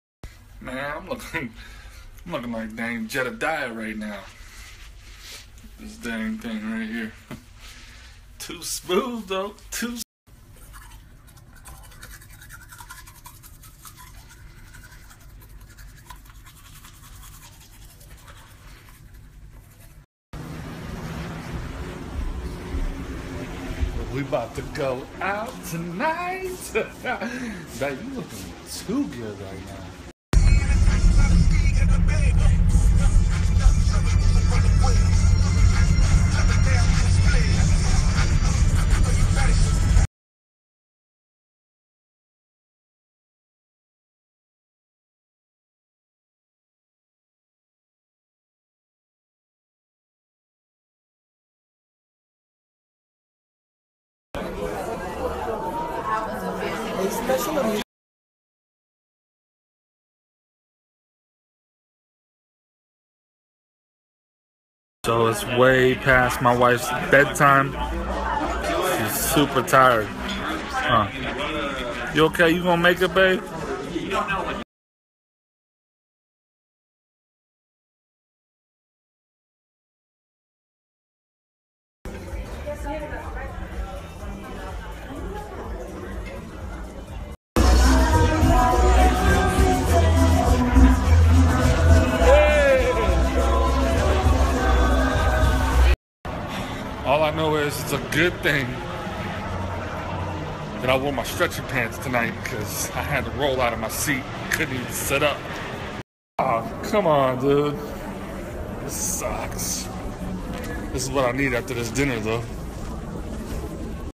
June 8th 2017. Celebrating our anniversary a day early.
My wife and I have been married for 8 and half years.

We didn't make a big deal out of this anniversary since we are going to universal studios at the end of the month.

We decided to go out and to dinner at BLT steak inside if Ballys hotel.